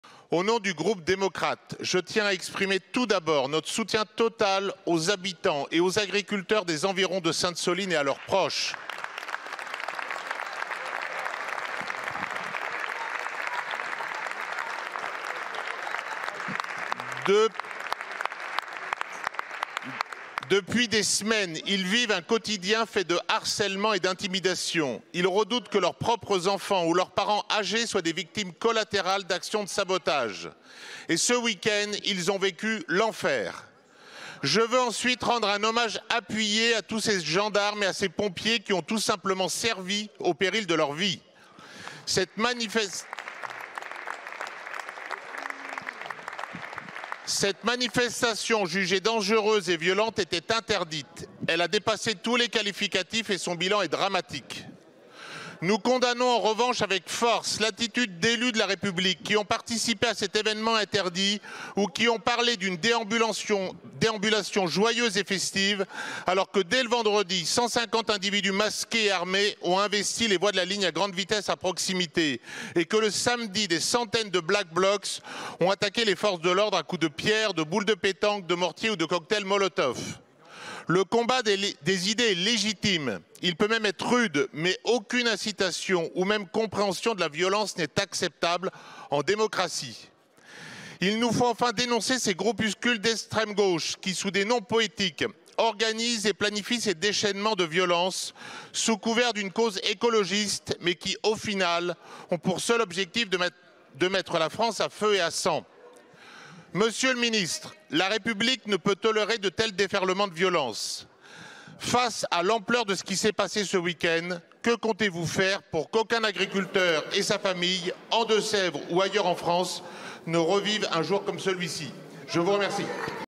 Aucune incitation ou excuse à la violence n'est acceptable en démocratie | Nicolas Turquois
Nicolas Turquois, Député MoDem de Vienne et agriculteur, est intervenu en séance de Questions au Gouvernement à l'Assemblée nationale ce mardi 28 mars à la suite des nouveaux affrontements violents autour du projet de megabasine à SainteSoline.